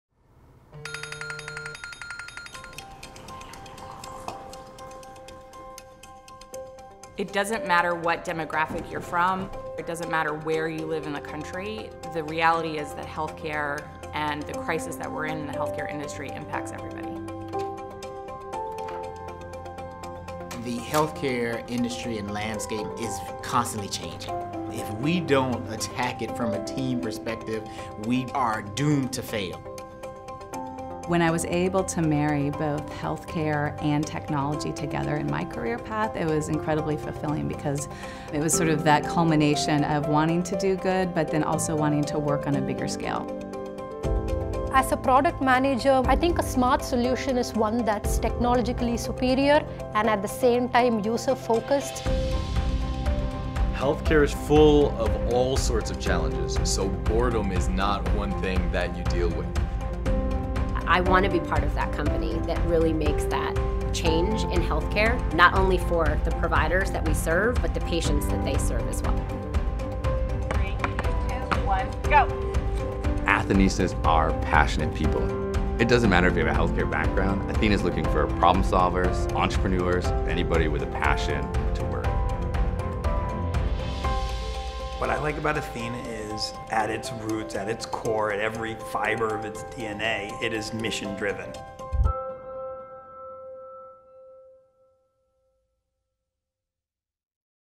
why athenahealth | athenahealth careers
Here at athenahealth, our work is meaningful, and everyone has a unique story about what being an athenista means to them. Hear from employees in five of our office locations about why they chose athenahealth.